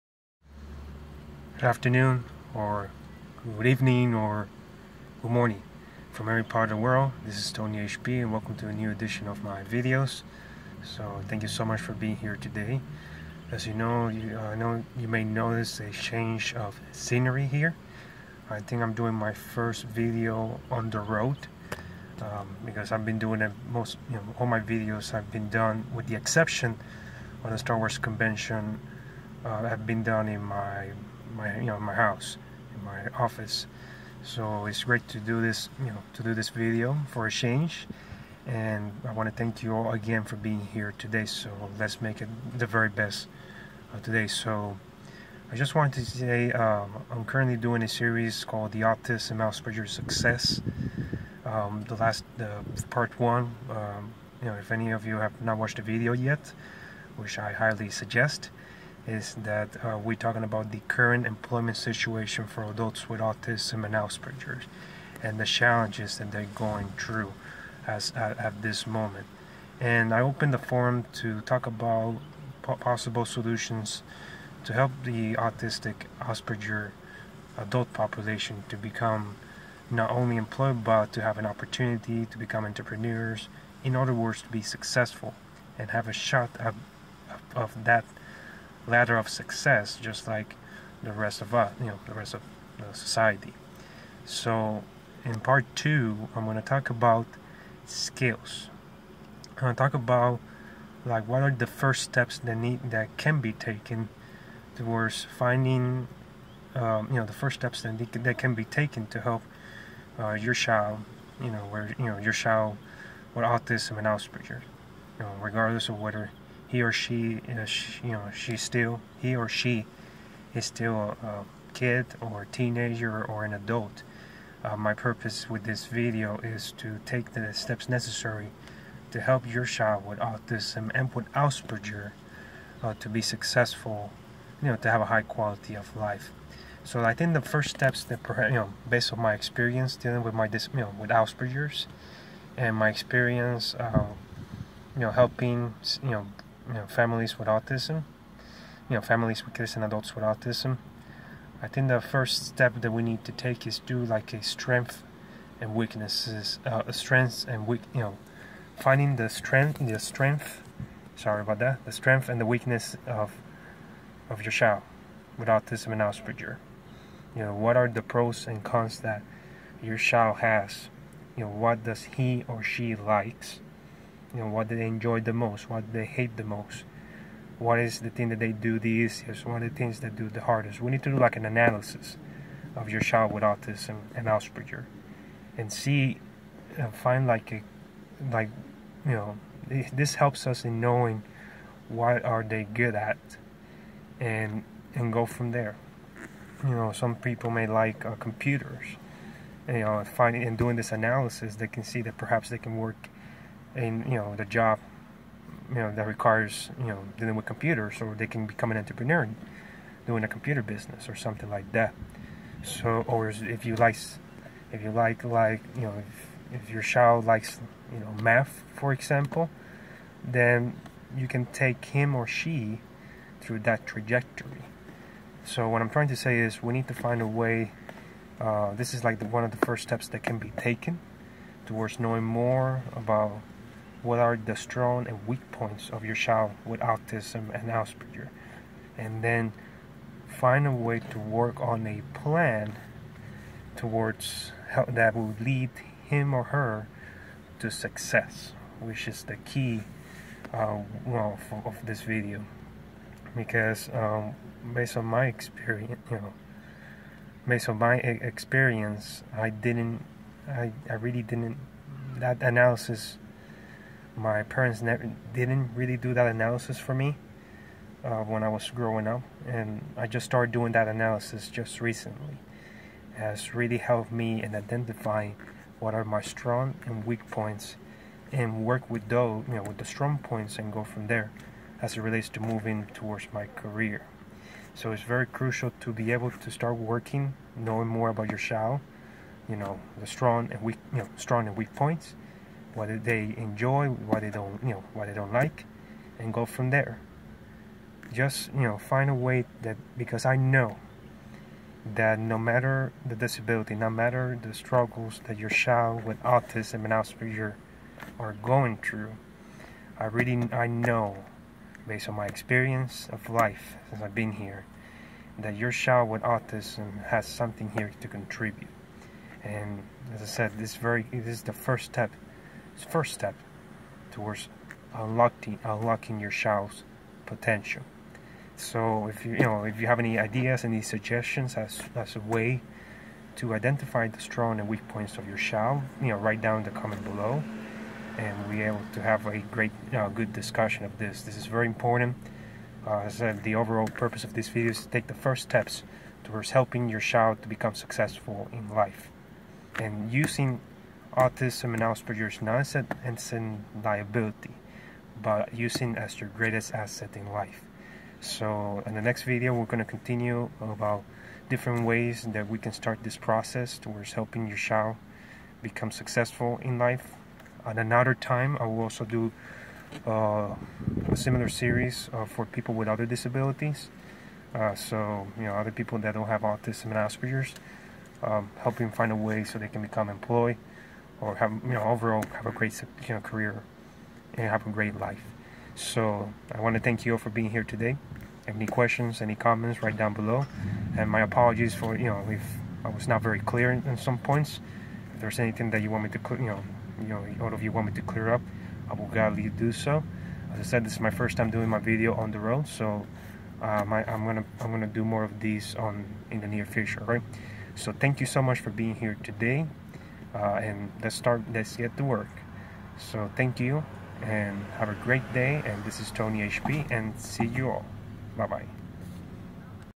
Autism /Asperger Success Part 2: SW Analysis
Here I talk about taking first steps towards helping people with autism Asperger towards having a successful life. First step: identifying their strengths and weaknesses.